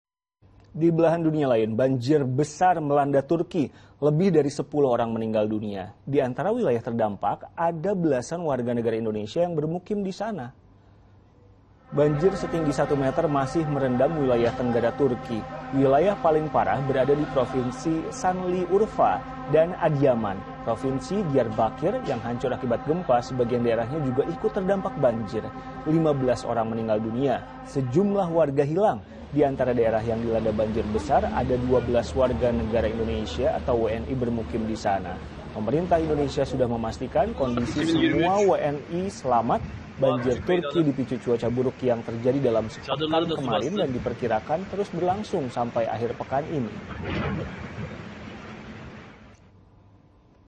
Turki Dilanda Banjir, 15 Orang Meninggal Dunia | Liputan 6
Beralih ke mancanegara. Banjir besar melanda Turki, sebanyak 15 orang meninggal dunia. Di antara wilayah terdampak, terdapat belasan warga negara Indonesia yang bermukim di sana.